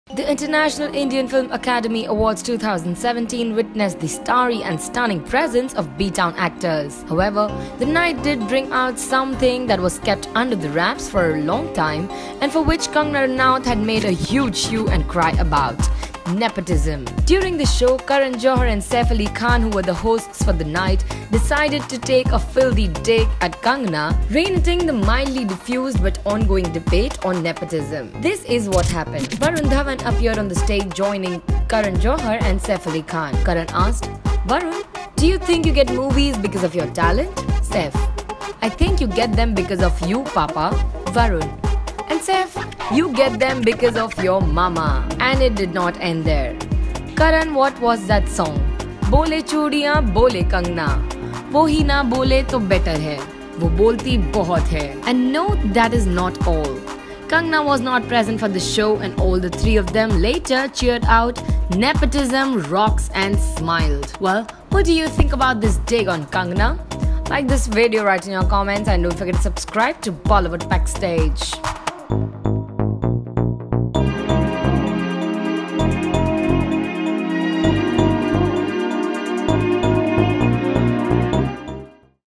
Saif, Varun & KJo take a DIG at Kangana Ranaut at IIFA Awards 2017 & its BAD!
The high and mighty of Bollywood were at their juvenile worst on Sunday. It was a shocking and sad moment during the recently-concluded IIFA 2017, where Karan Johar, Saif Ali Khan and Varun Dhawan ganged up at the New York stage of the festival and made fun of Kangana Ranaut’s nepotism statements.